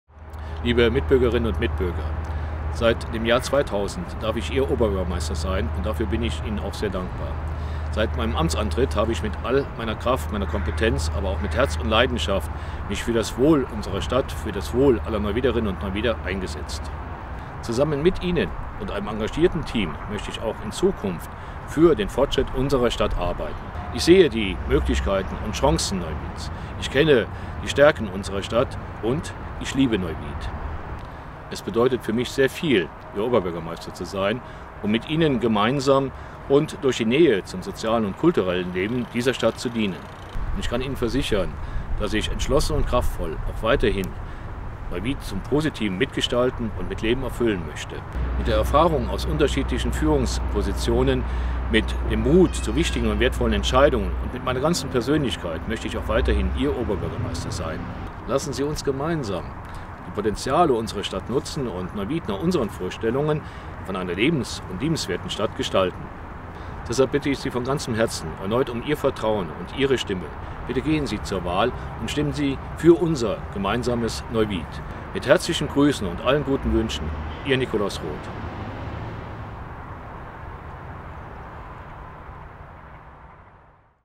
Nikolaus Roth - Stichwahl 22.11.2015
Bei der Oberbürgermeisterwahl in Neuwied hat Amtsinhaber Nikolaus Roth (SPD) das Rennen gegen drei Herausforderer am Sonntag, dem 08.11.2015 klar gewonnen.

Mit 48,8 Prozent der Stimmen verfehlte Roth die absolute Mehrheit aber knapp und muss sich nun einer Stichwahl stellen.

Quelle: focus.de / Inhalt bereitgestellt von dpa
Video: newcut werbefilme e.K.